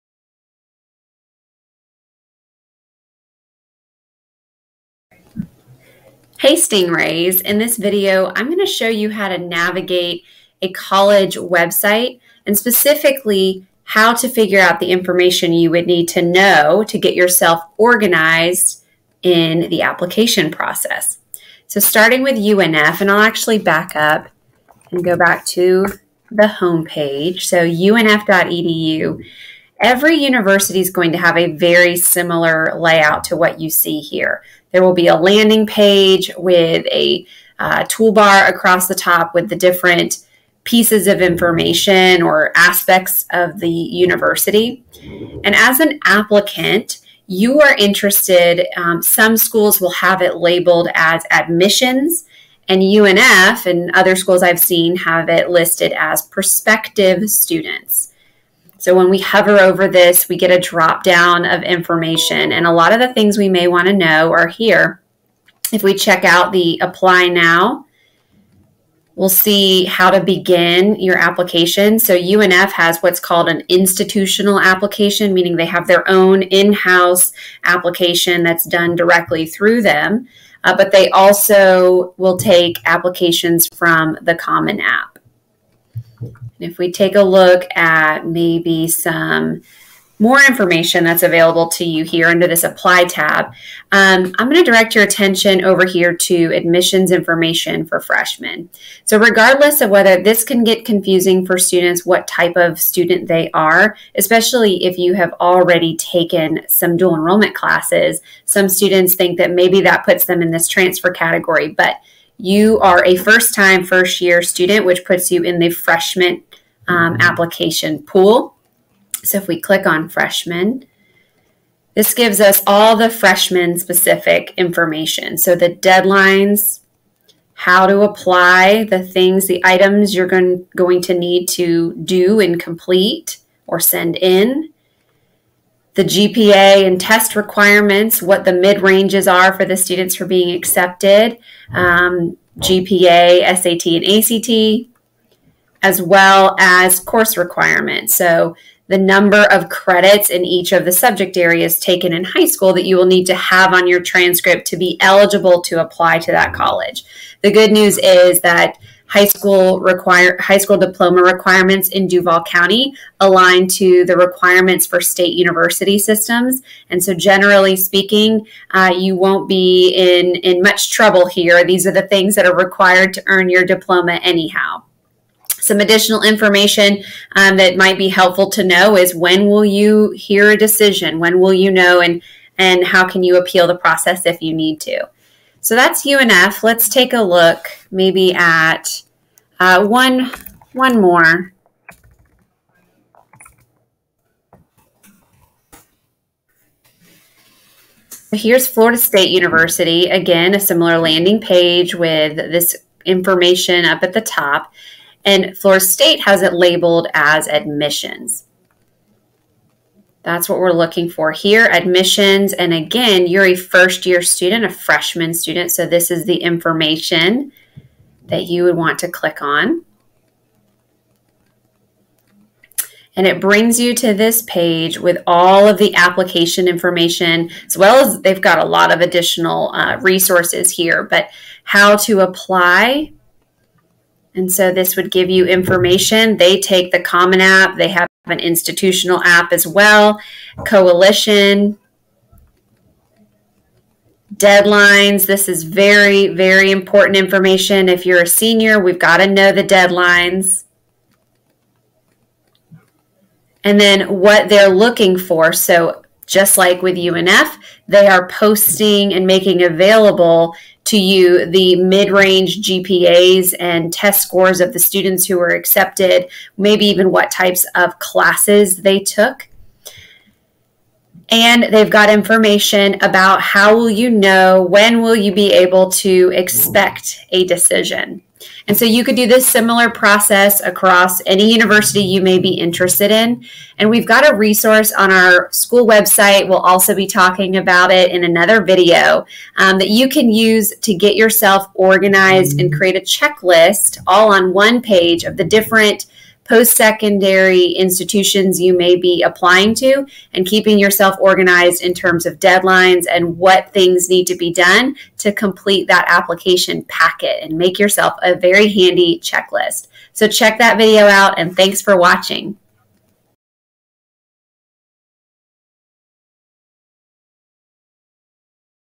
Navigating a College Website
Check out this video for an example of how to browse a college website to determine what steps must be taken to apply, and what the application requirements and deadlines are.